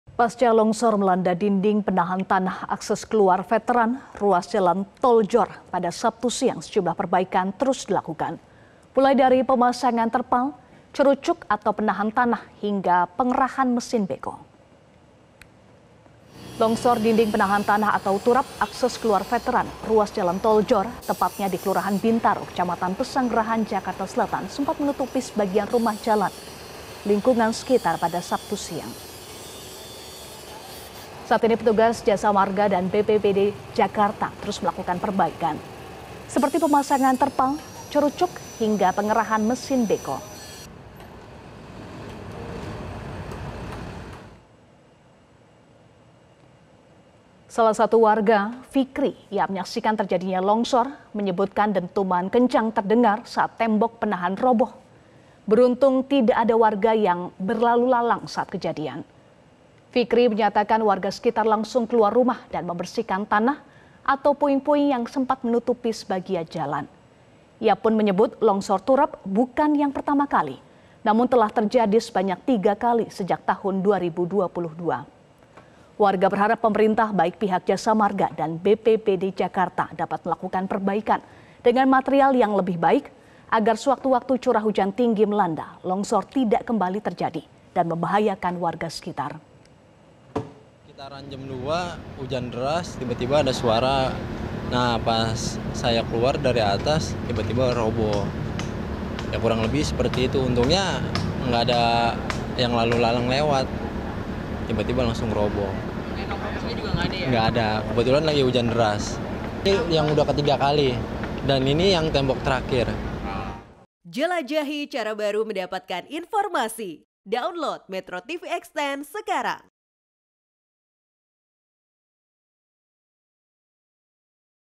UPDATE! PERBAIKAN LONGSOR TURAP TOL JORR VETERAN JAKSEL
MetroTV, Pasca longsor melanda dinding penahan tanah akses keluar Veteran, ruas jalan Toll JORR pada Sabtu siang, sejumlah perbaikan terus dilakukan. Mulai dari pemasangan terpal, cerucuk atau penahan tanah, hingga pengerahan mesin beko